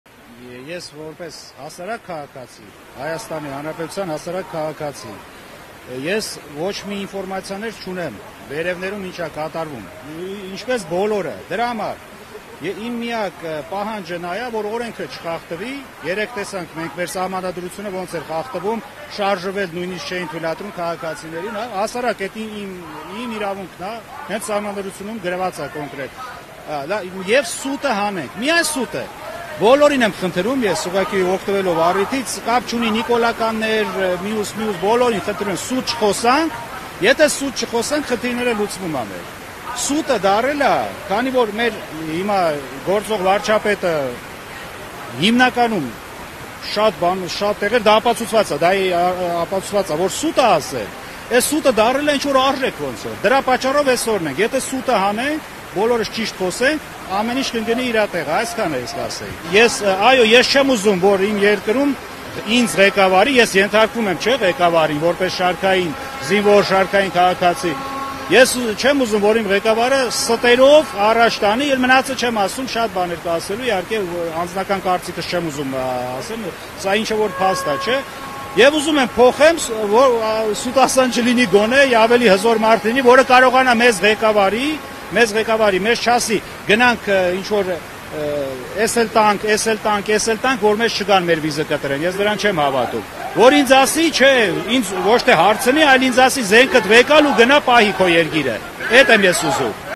Չեմ ուզում ինձ ղեկավարի մեկը, ով ասելու է՝ սա էլ տանք, որ չգան մեզ սպանեն. ակցիայի մասնակից
Գործող վարչապետի ստերի պատճառով է, որ այսօր հասել ենք այստեղ: Այս մասին Alpha News-ին Կառավարության շենքի մոտ ասաց ակցիայի մասնակիցներից մեկը:

«Ես որպես Հայաստանի Հանրապետության քաղաքացի պահանջում եմ, որ օրենքը չխախտվի: Երեկվա օրը օրենքի խախտման վառ օրինակ է, մարդկանց թույլ չէին տալիս անգամ մի տեղից մյուսը գնալ: Ես բոլորին խնդրում եմ՝ եկեք սուտը հանենք մեր միջից, կապ չունի՝ ինչ քաղաքական հայացքներ ունեք, ուղղակի եկեք չստենք միմյանց: Չստելով մեր բոլոր խնդիրները կլուծվեն: Մեր գործող վարչապետի ստերի պատճառով է, որ այսօր հասել ենք այստեղ, ունենք այն ինչ ունենք: Ես չեմ ուզում իմ երկիրը ղեկավարի մի մարդ, ով իր ուզածին հասնում է միայն սուտ խոսելով: Եվ այո, միանում են վարչապտի հրաժարականի պահանջին: Չեմ ուզում ինձ ղեկավարի մեկը, ով ասելու է սա էլ տանք, որ չգան մեզ սպանեն»,- ասաց ակցիայի մասնակիցը: